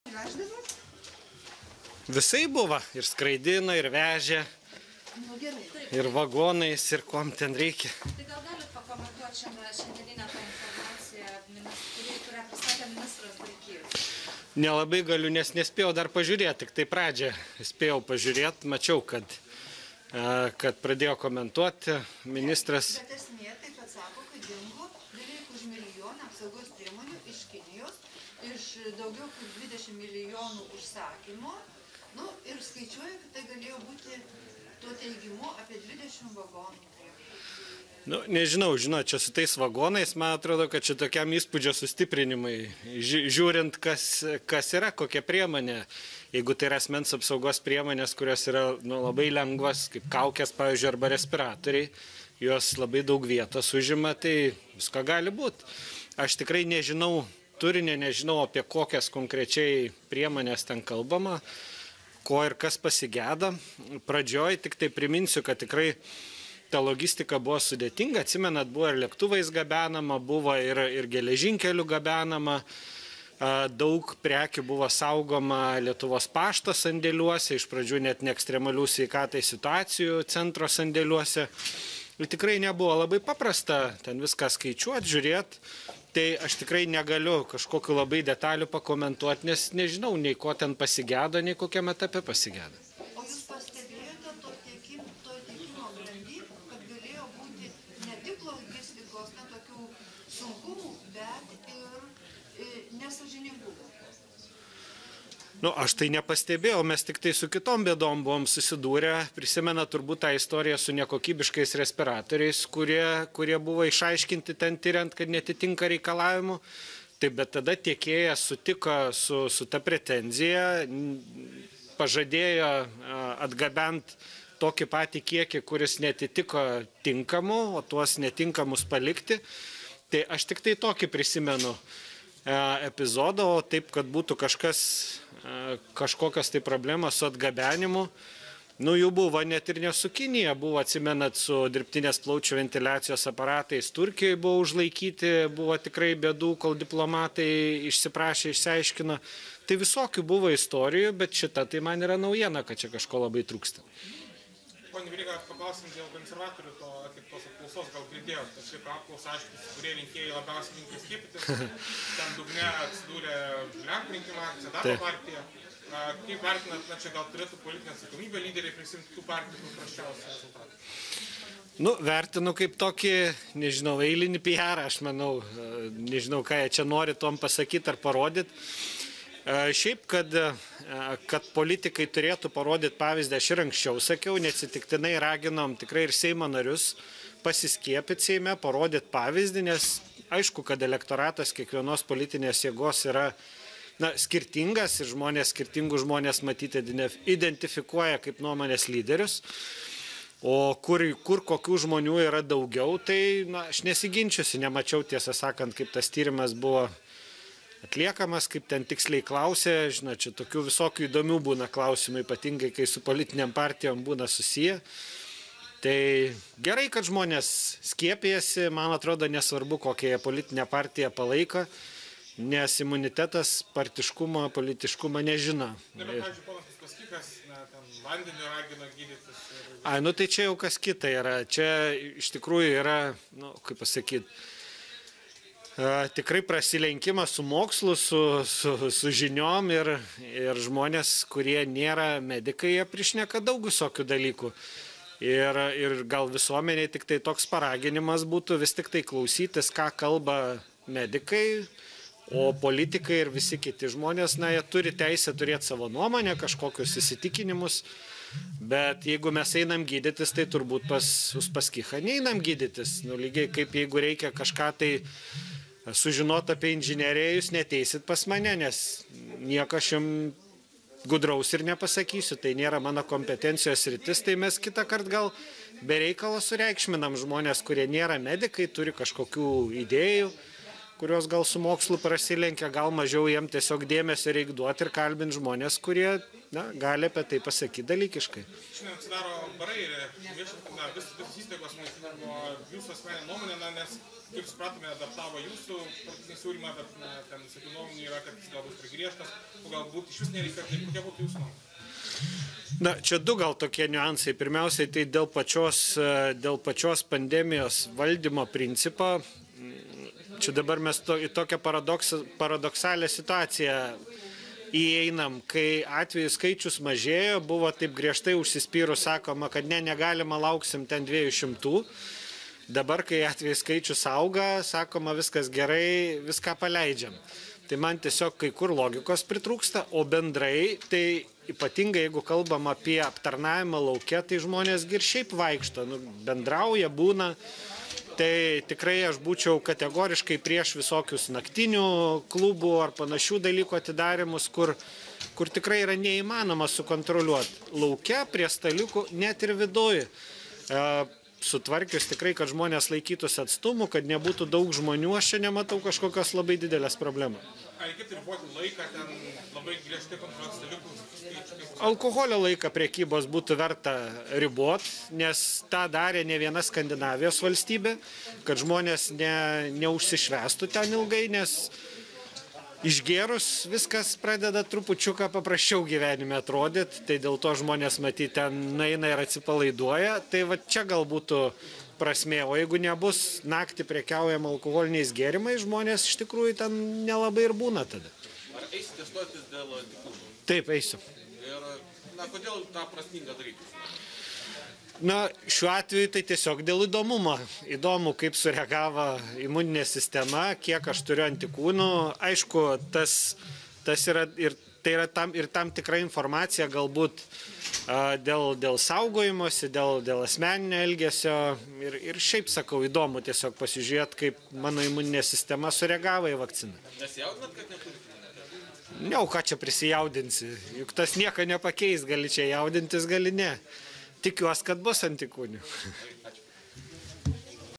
A. Veryga apie iš SAM panosės dingusias apsaugos priemones: nežinau, ko ir kas pasigedo
Buvęs sveikatos apsaugos ministras Aurelijus Veryga sako neturintis žinių, kad būtų galėjusios dingti apsaugos priemonės, tačiau pažymi, kad logistikos procesas pandemijos pradžioje buvo sudėtingas. „Aš tikrai nežinau turinio, nežinau, apie kokias priemones kalbama, ko ir kas pasigedo, pradžioj tik priminsiu, kad tikrai logistika buvo sudėtinga. Buvo lėktuvais gabenama, geležinkeliu, daug prekių buvo saugoma Lietuvos pašto sandėliuose, iš pradžių net ne Ekstremalių situacijų centro sandėliuose“, – ketvirtadienį žurnalistams Seime sakė A. Veryga. „Tikrai nebuvo labai paprasta viską skaičiuot, žiūrėt, todėl negaliu detalių pakomentuot, nes nežinau, nei ko ten pasigedo, nei kokiam etape pasigesta“, – teigė buvęs ministras. Sveikatos apsaugos ministerija ketvirtadienį pranešė, jog kreipėsi į teisėsaugą dėl apsaugos priemonių trūkumo. Anot sveikatos apsaugos ministro Arūno Dulkio, trūksta apsaugos priemonių už 912 tūkst. eurų.